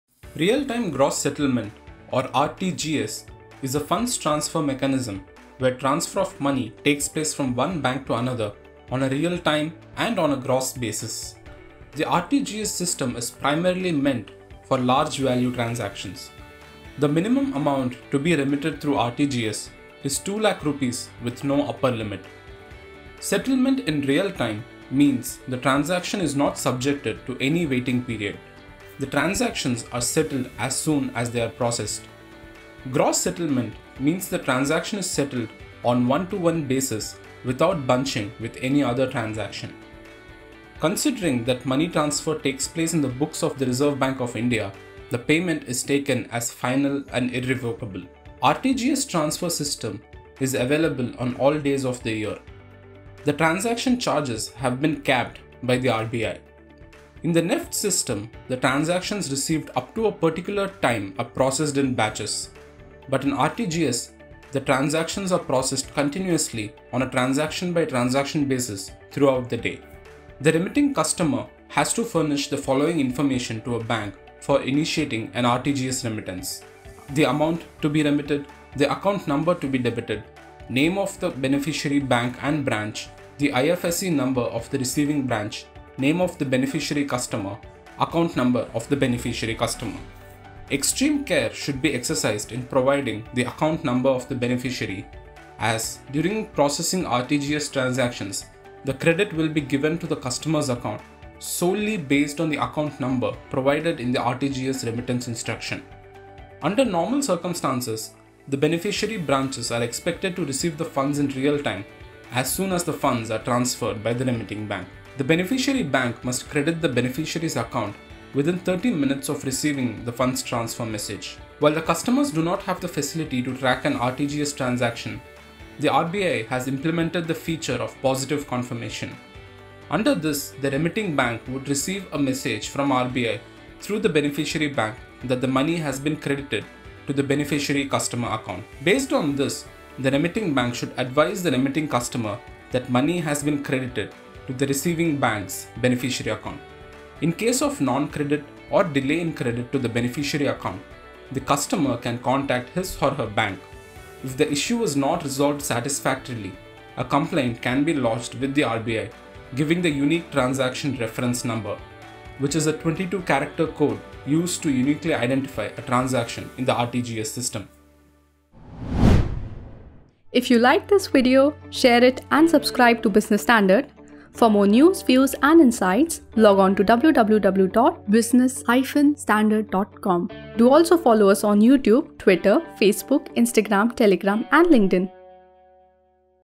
What is RTGS funds transfer system?
Real Time Gross Settlement or RTGS system has cut short the risk to high-value payment settlements among financial institutions. It is used by central banks worldwide. Find out more about the system.